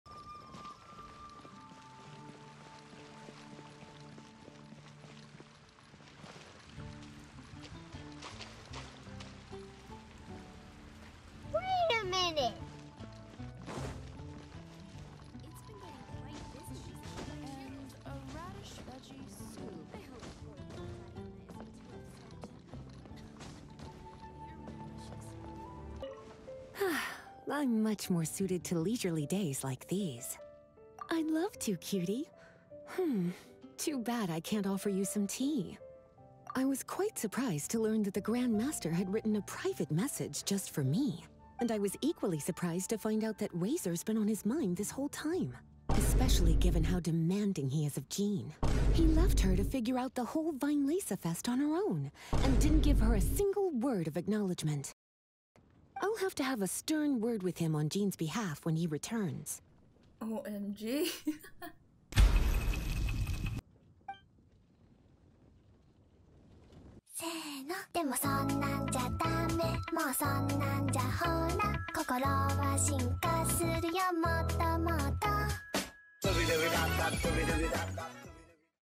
Varka is no exception to Lisa 💀
If there's one character im gonna be scared of, it's gonna be Lisa XD
Imagine, even if Varka is the Grandmaster and is older than her, she will DEFINITELY scold anyone and choose violence when she sees fit. 💀

*link will only be available during live stream and will be deleted after the stream. So join right away lol*